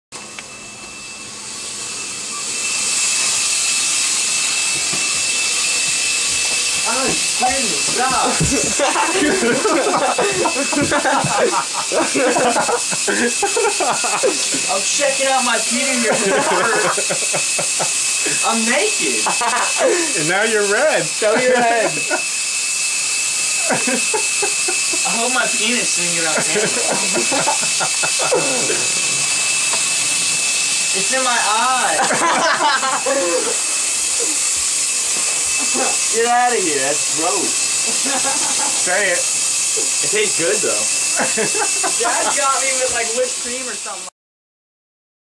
Kool Aid Shower
naked kid gets some kool aid with his shower